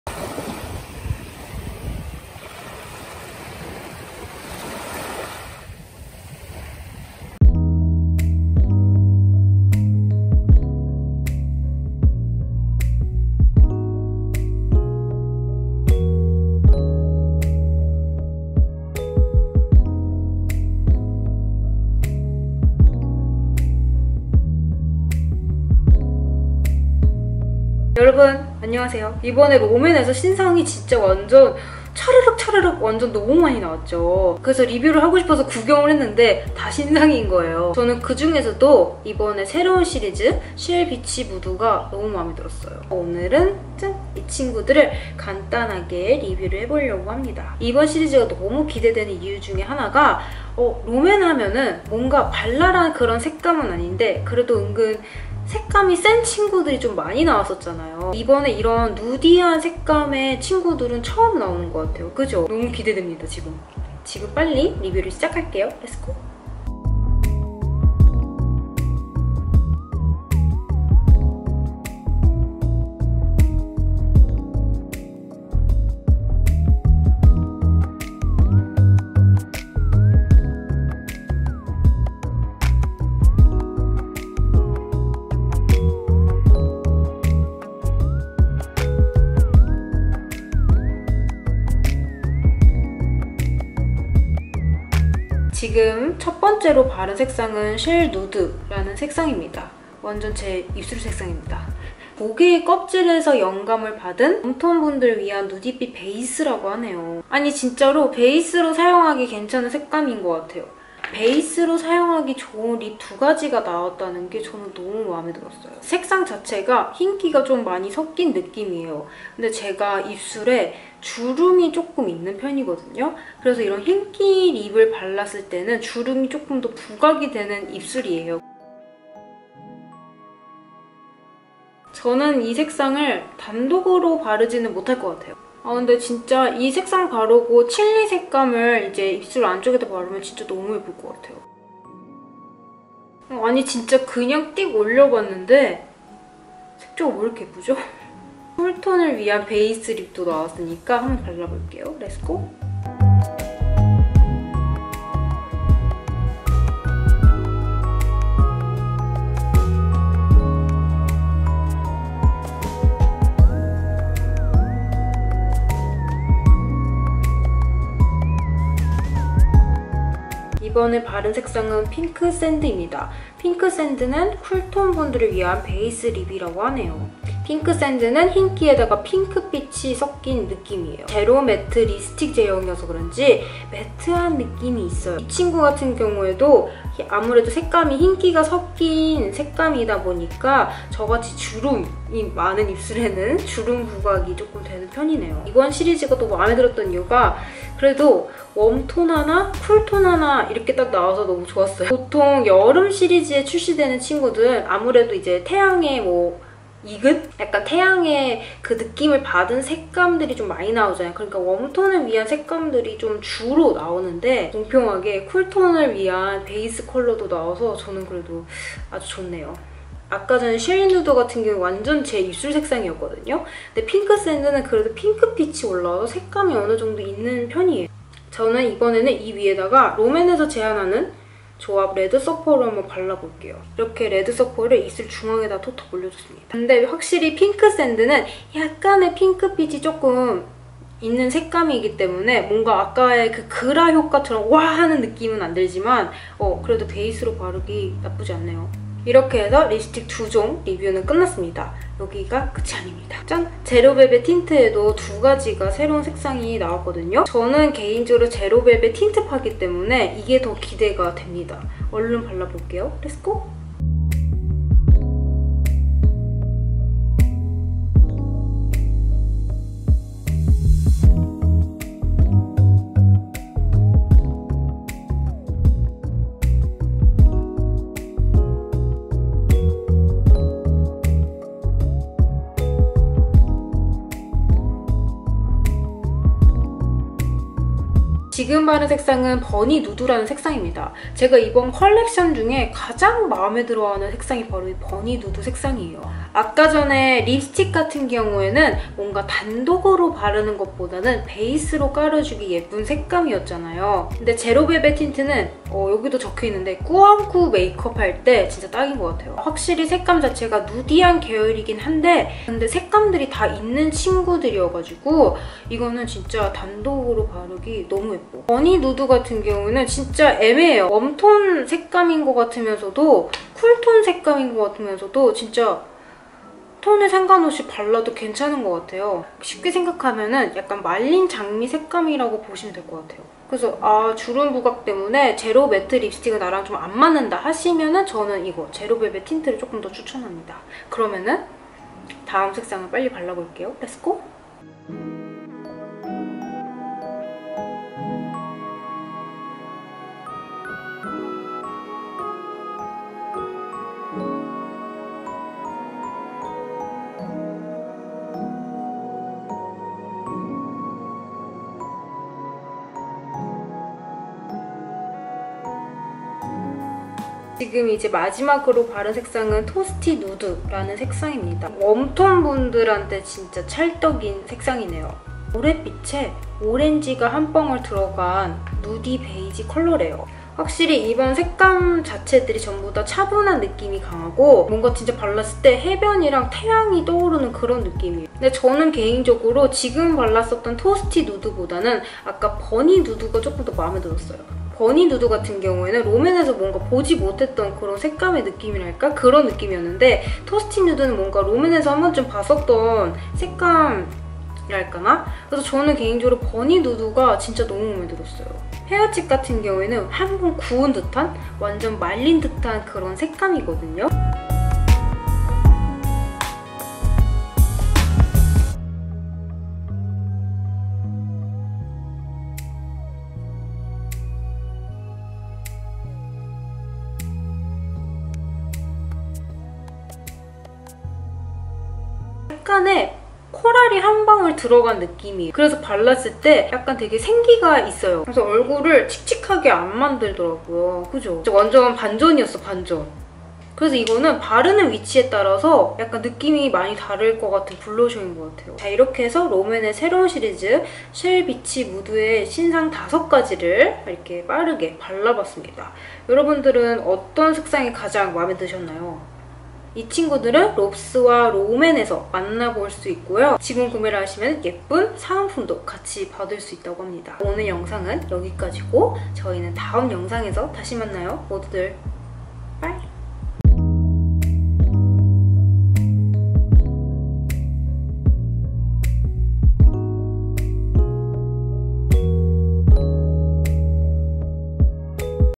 (ENG)🏖롬앤 20S/S신상 쉘비치누드⛵️ 5종 간단리뷰/ROMAND Shell Beach Nude Review/Lilac라일락
안녕하세요 라일락입니다:]
이번에 롬앤에서 신제품이 우르르ㅡ르륵 출시되었죠
제로매트립스틱 제로벨벳틴트 베러댄치크 등등 정말 다양하게 출시가 되었는데요, 오늘은 쉘비치누드 컬렉션을 가지고 왔습니다!
여러분들은 이번 신상 중에서 어떤 제품이 가장 마음에 드시나요?!

*본 제품은 롬앤으로부터 제품을 제공받았습니다.

LIP
Romand Zero matte lipstick SHELL NUDE
롬앤 제로매트립스틱 쉘누드

Romand Zero matte lipstick PINK SAND
롬앤 제로매트립스틱 핑크샌드

ROMAND Zero velvet tint BURNY NUDE
롬앤 제로벨벳틴트 버니누드

Romand Zero velvet tint TOASTY NUDE
롬앤 제로벨벳틴트 토스티누드

Romand Better than cheek PEAR CHIP
롬앤 베러댄치크 페어칩